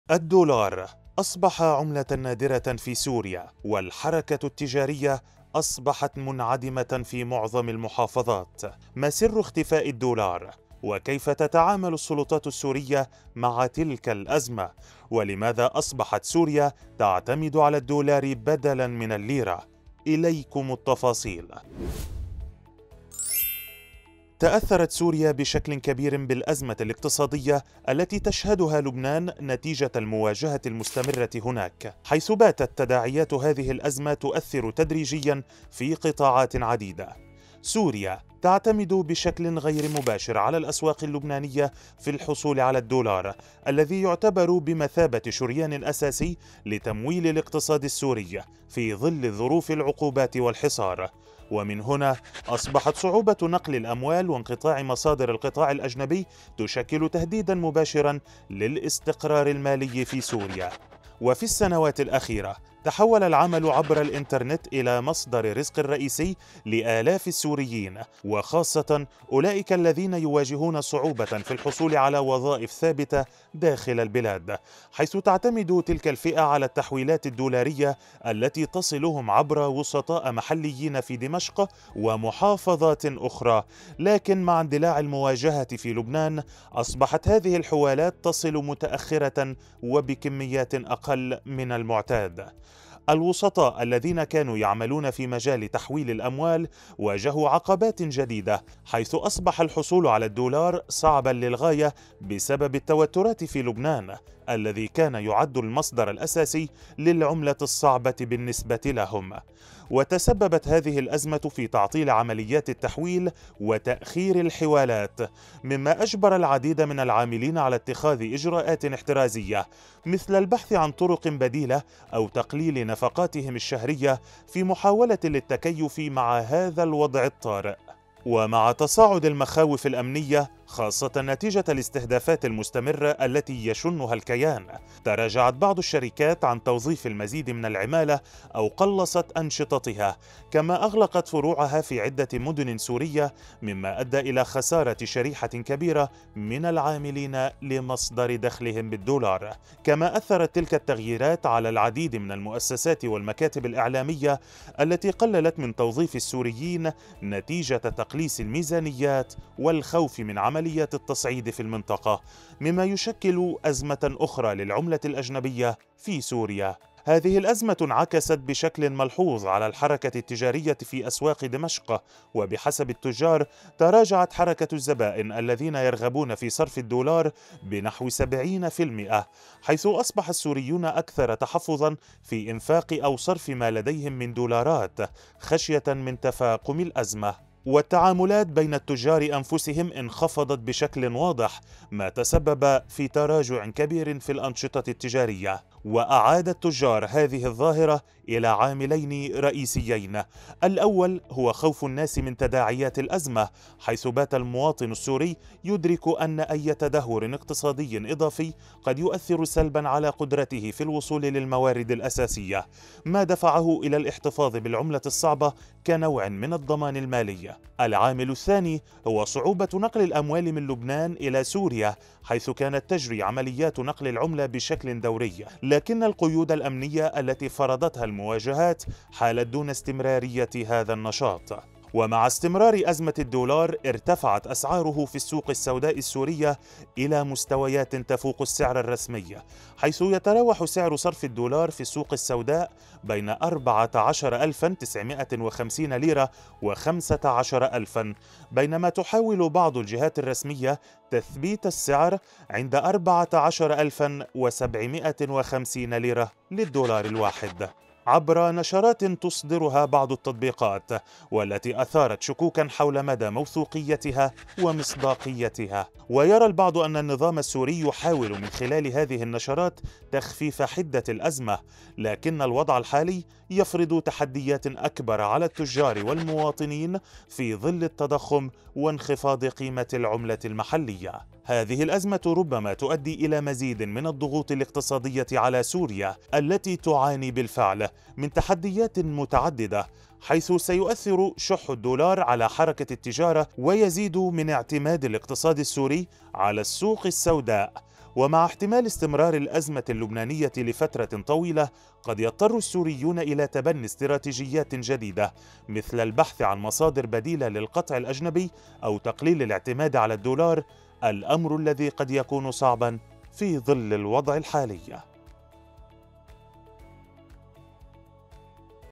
الدولار يختفي في السوق السورية.. سعر الليرة يصل لرقم غير مسبوق.. لماذا يحتفظ المواطنين بالأموال ؟
تأثرت سوريا بشكل كبير بالأزمة الاقتصادية التي تشهدها لبنان نتيجة المواجهة المستمرة هناك، حيث باتت تداعيات هذه الأزمة تؤثر تدريجيا في قطاعات عديدة.
سوريا تعتمد بشكل غير مباشر على الأسواق اللبنانية في الحصول على  الدولار .